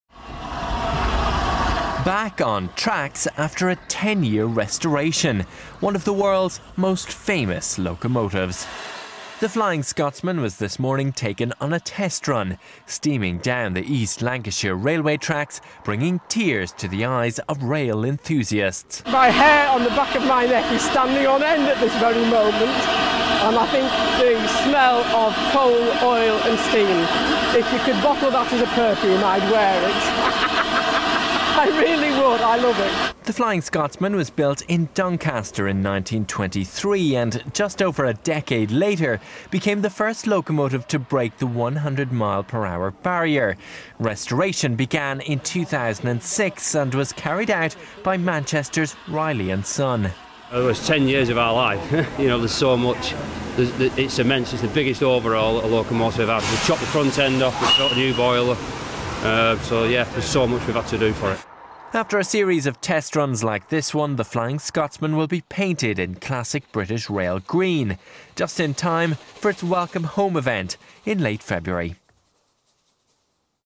Flying Scotsman flies again: Steam locomotive returns
The Flying Scotsman brought tears to the eyes of train enthusiasts when it returned to the tracks after a decade-long refit. Report by Conor Mcnally.